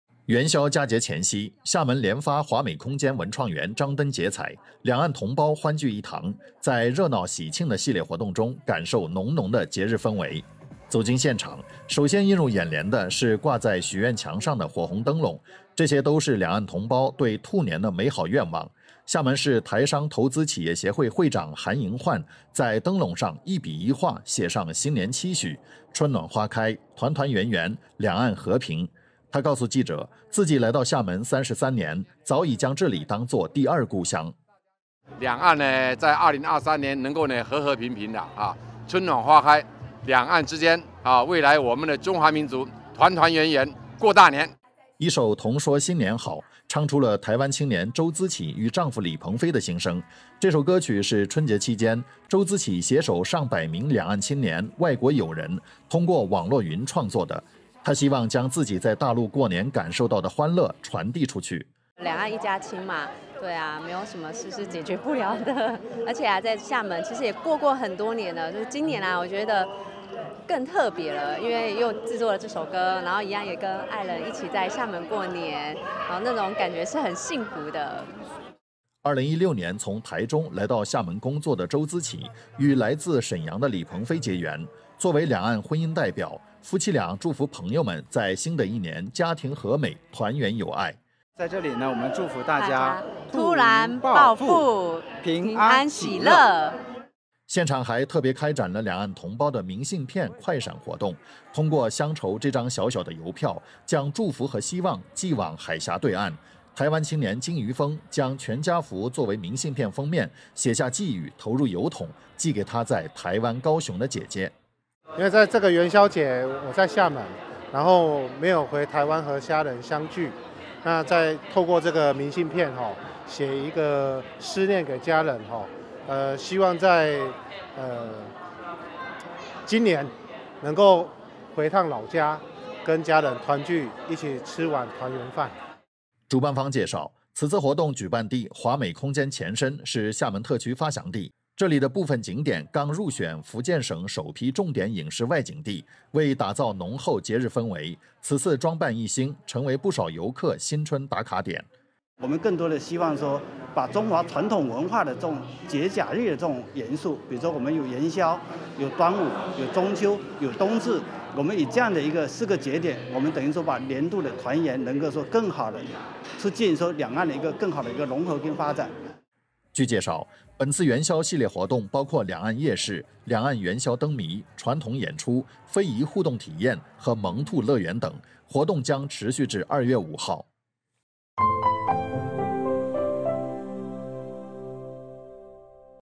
两岸同胞张灯结彩“闹元宵” 人间至味是团圆
元宵佳节前夕，厦门联发华美空间文创园张灯结彩，两岸同胞欢聚一堂，在热闹喜庆的系列活动中，感受浓浓的节日氛围。